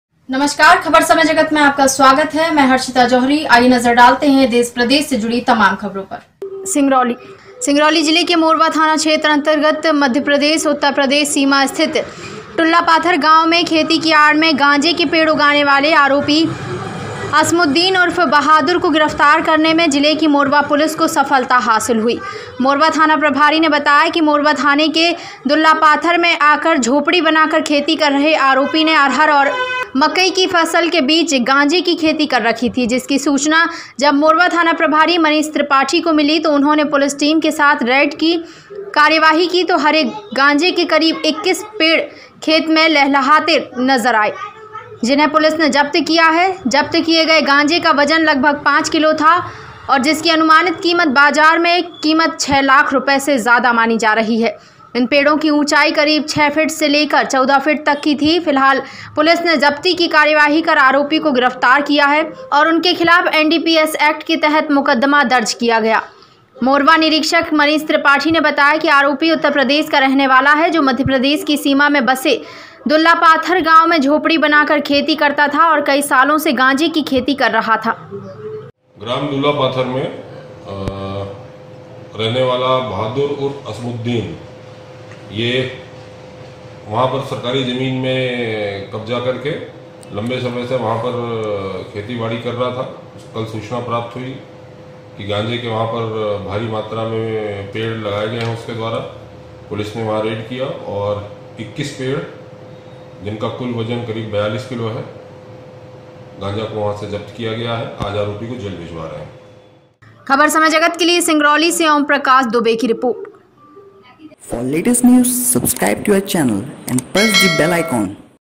सिगंरौली - खेती की आड़ में 6 लाख का गांजा के पेड़ उगाने वाले आरोपी असमुद्दीन उर्फ बहादुर को गिरफ्तार
सिगंरौली से ओम प्रकाश दुबे